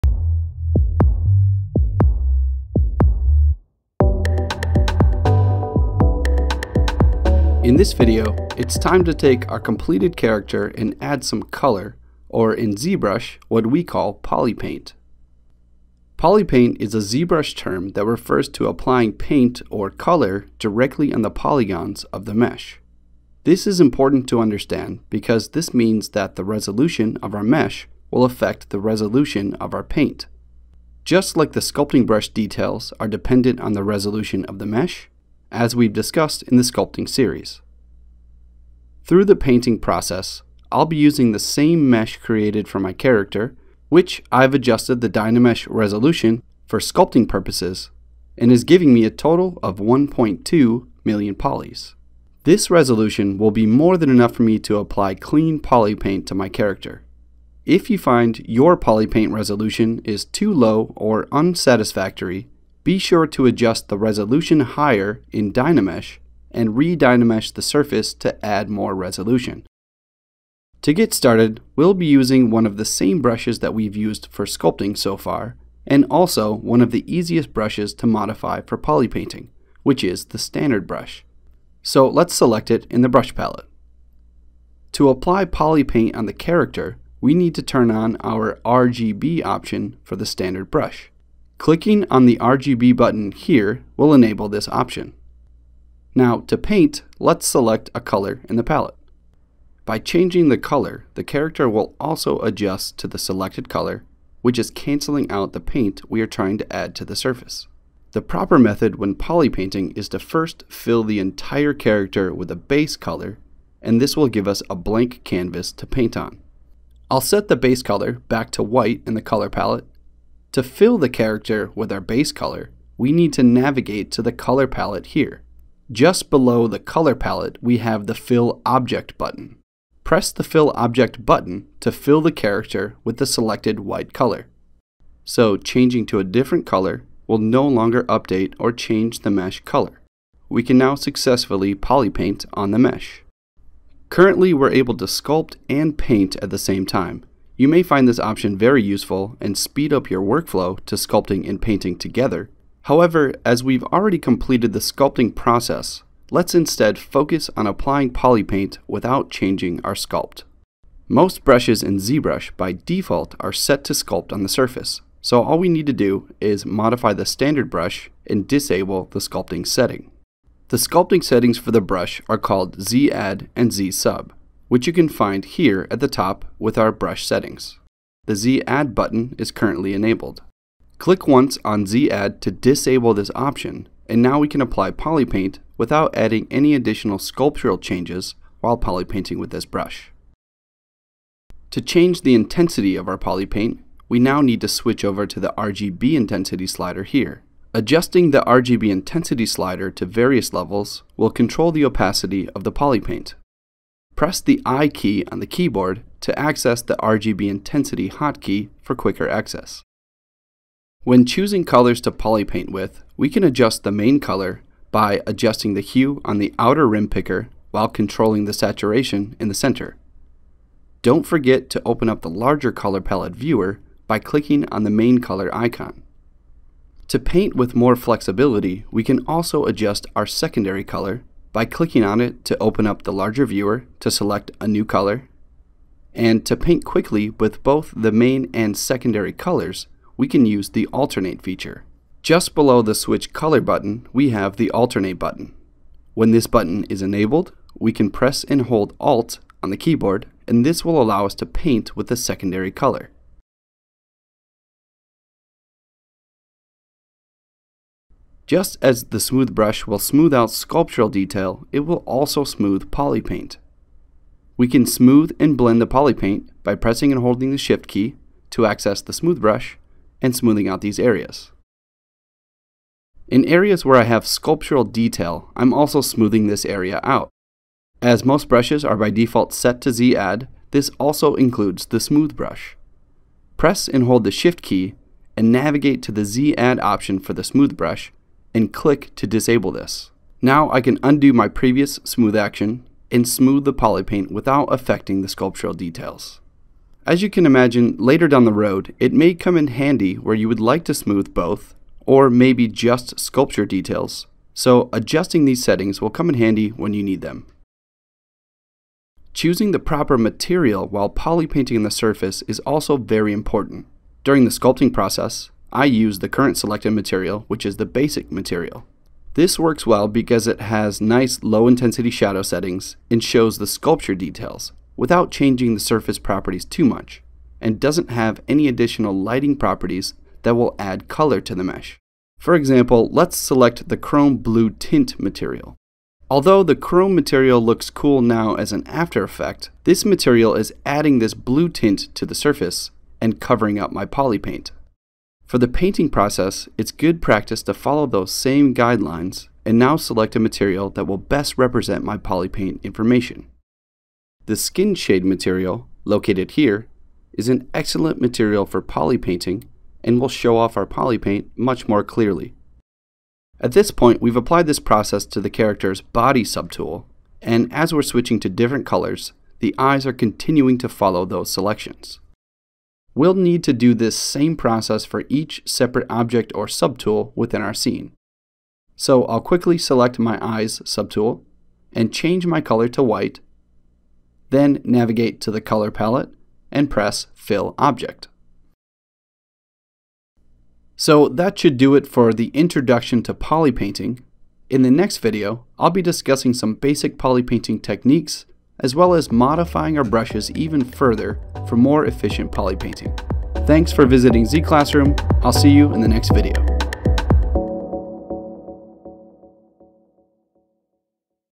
How to Start with ZBrush - PolyPaint Basics - Part 10
This video introduces the basics of applying PolyPaint to the character in ZBrush.